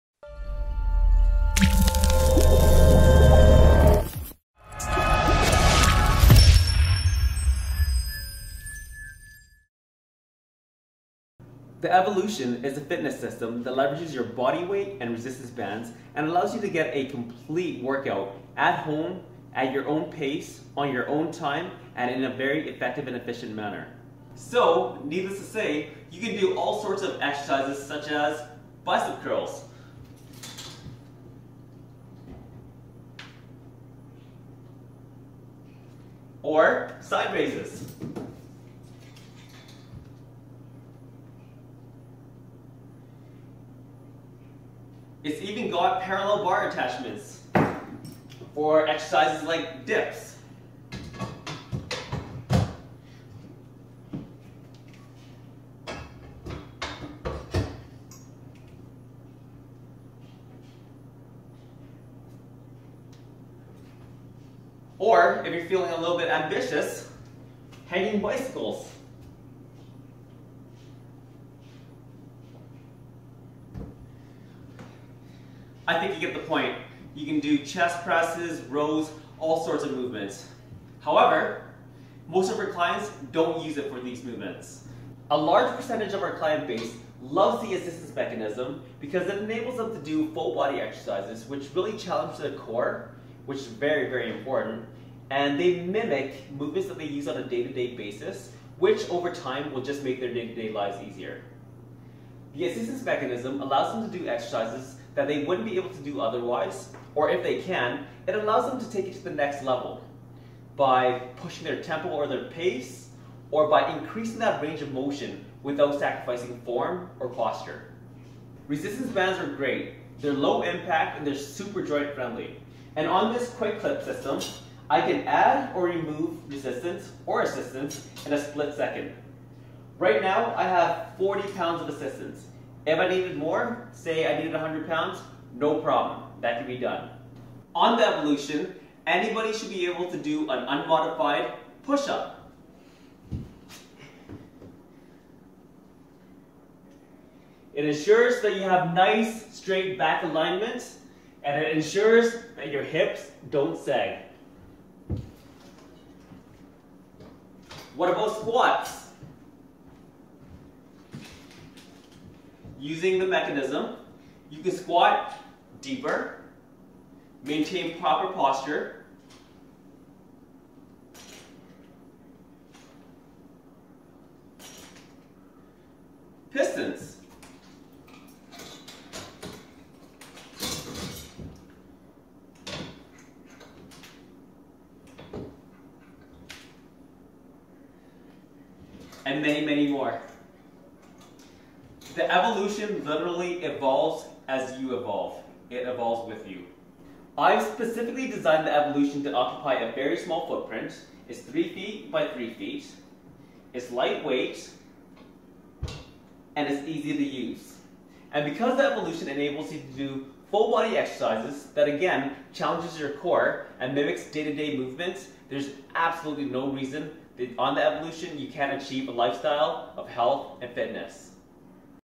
What is the Evolution All-In-One Training System
The Evolution is the most versatile home fitness machine which makes exercise fun, practical, and, most of all, convenient, for the busy individual.